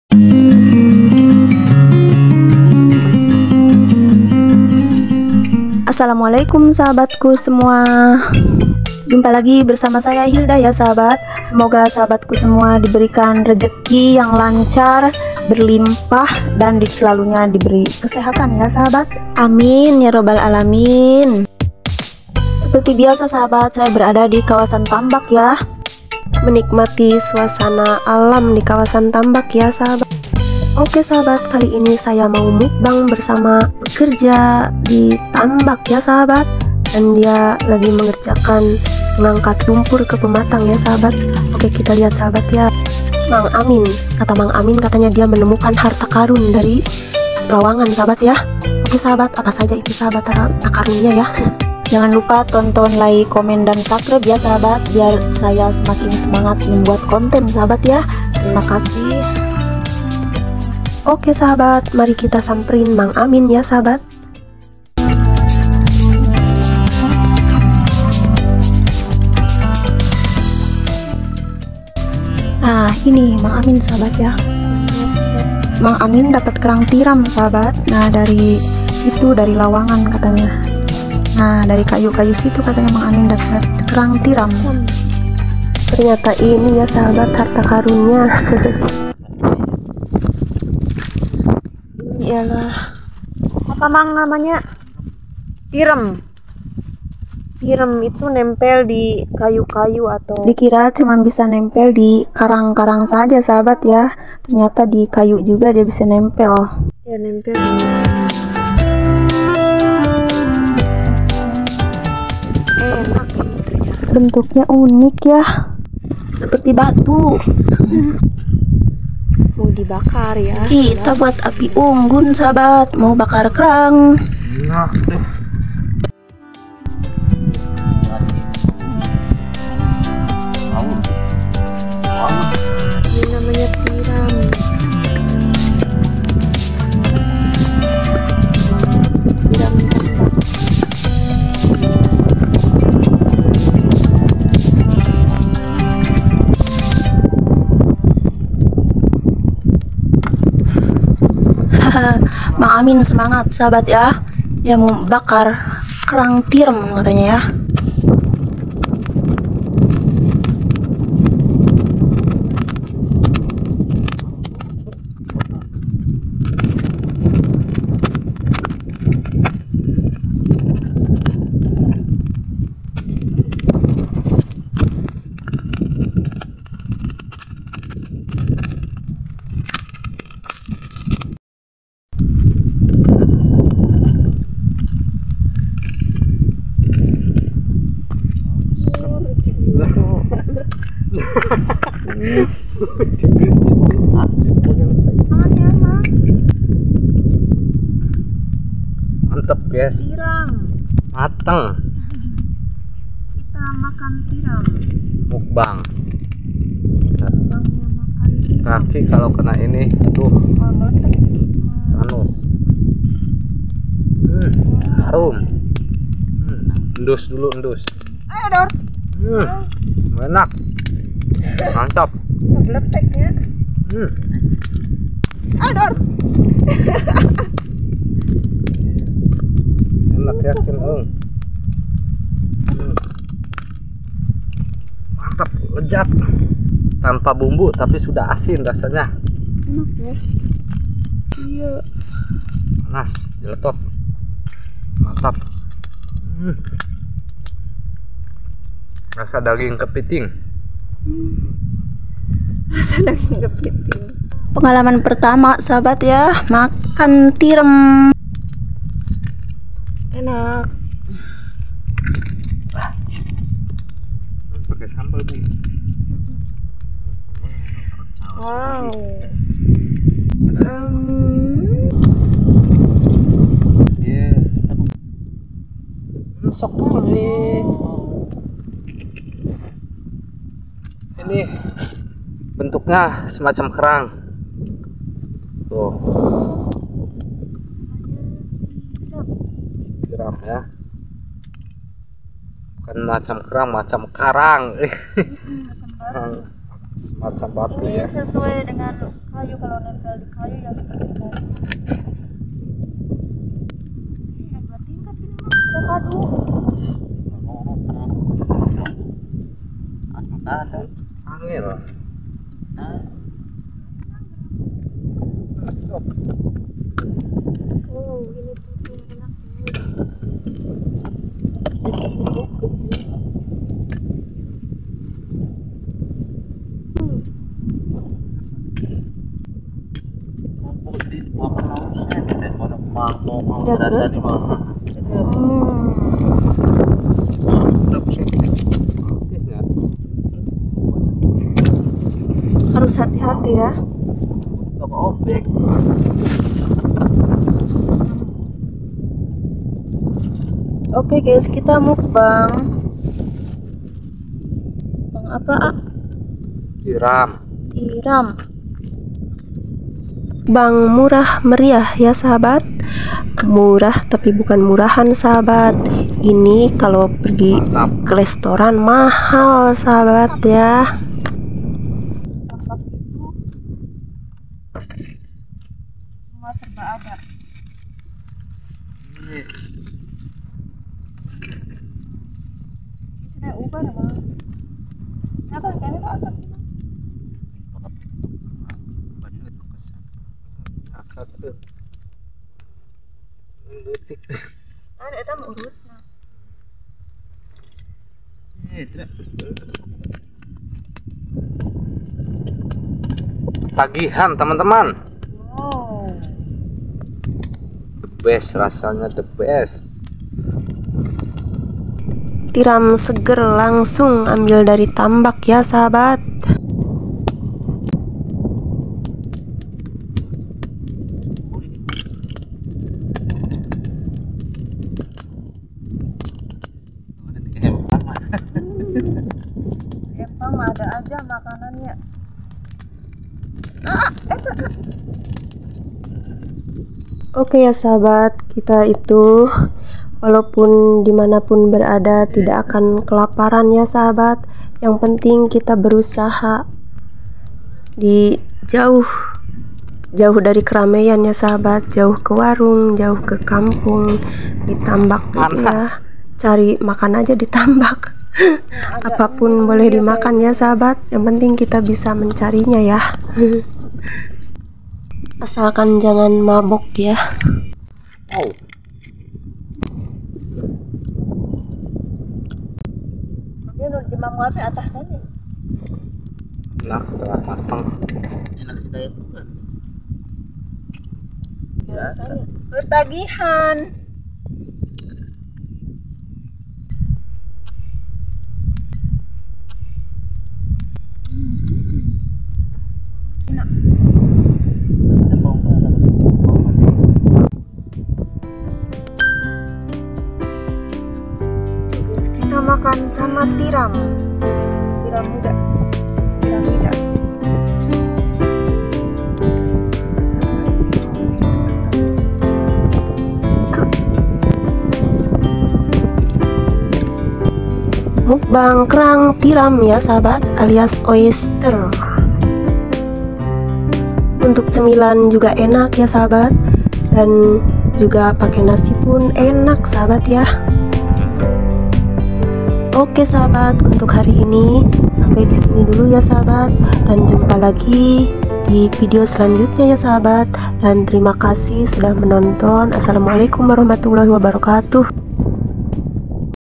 mencari kerang tiram ditambak bandeng||auto mukbang || vlog 019
Menikmati suasana alam di kawasan Tambak @Hildarantau

kali ini bersama mang amin mukbang kerang tiram segar langsung di ambil dari Tambak dan di bakar..
kerang tiram bakar rasanya bikin nagih,berburu kerang mahal secara gratis
Rejeki nomplok menemukan sarang kerang tiram di tambak
pengalaman pertama makan kerang tiram atau oiyster..
makanan dari Tambak gratis,